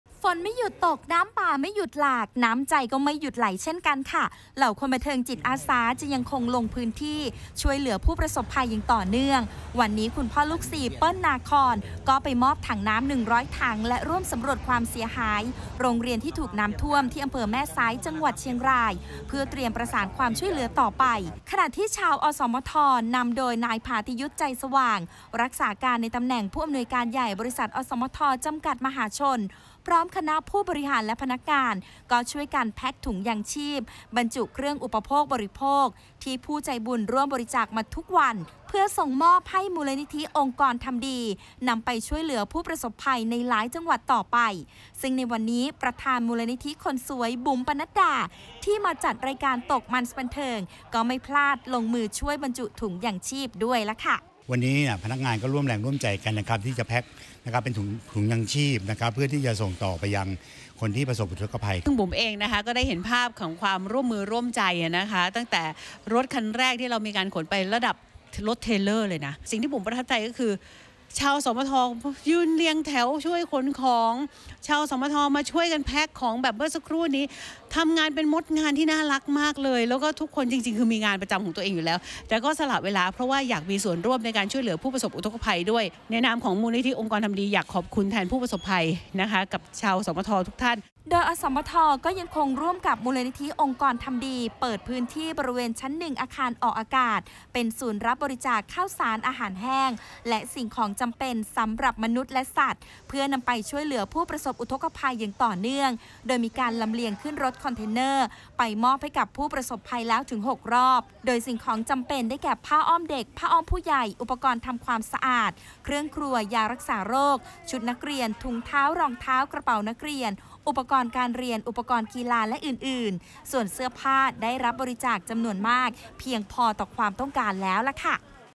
“อสมท” แพ็กถุงยังชีพส่งมอบมูลนิธิองค์กรทำดี
คณะผู้บริหารและพนักงาน บริษัท อสมท จำกัด (มหาชน) ช่วยกันแพ็กถุงยังชีพบรรจุเครื่องอุปโภคบริโภคที่ผู้ใจบุญร่วมบริจาคมาทุกวัน เพื่อส่งมอบให้มูลนิธิองค์กรทำดี นำไปช่วยเหลือผู้ประสบภัยในหลายจังหวัดต่อไป